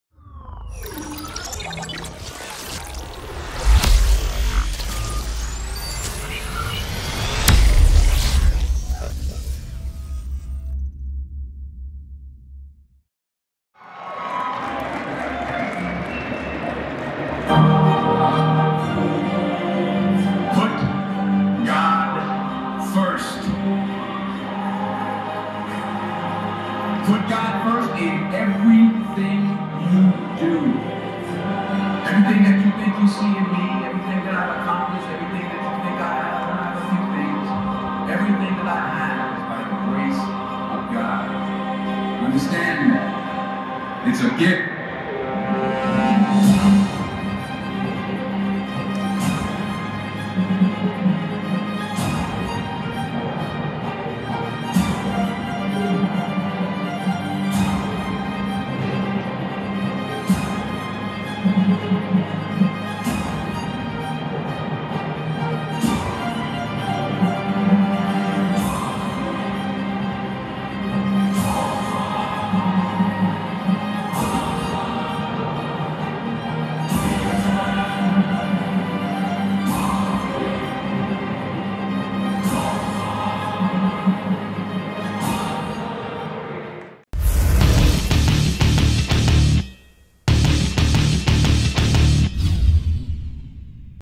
Harold Kelley's Routine - 2022 Wheelchair Olympia
2022 Wheelchair Olympia
December 17, 2022
Las Vegas, Nevada - USA